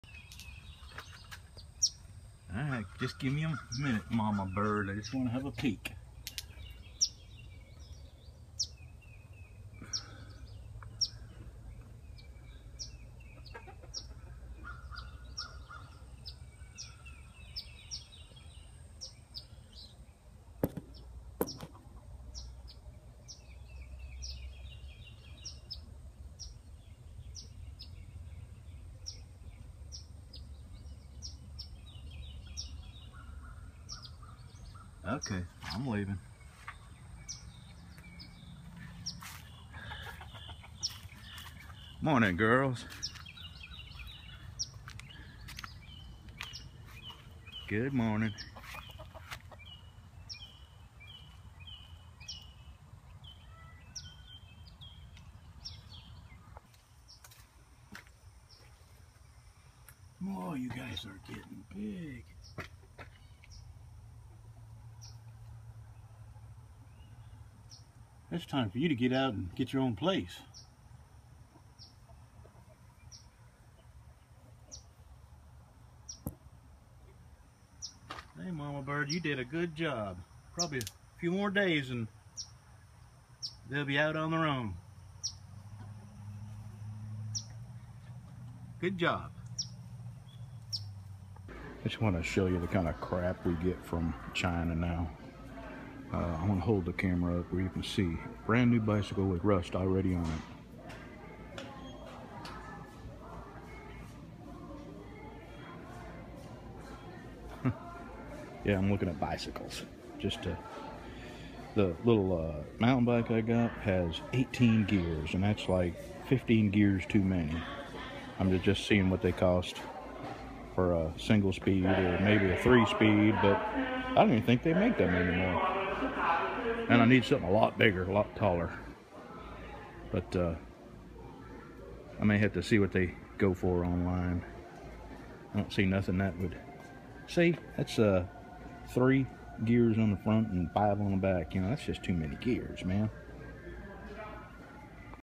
The baby birds are gone!
I checked this morning and the babies have all flown away. The 18 speed mountain bike that I bought for ten bucks,has to many gears and isn't tall enough. I sold it for 50 bucks after fixing the front derailer. Found a bike at Walmart for 120 bucks. The seat had a tear in it and I asked the manager to knock off 20 bucks and the did. I had 50 bucks on my Walmart savings catcher card and 50 bucks that I sold the other bike for and I had to pay 9 bucks out of my own pocket. So far I'm riding it 1.5 miles a day. 3/4 of a mile in the morning and again in the evening. I hope to work my way up to 3 miles a day.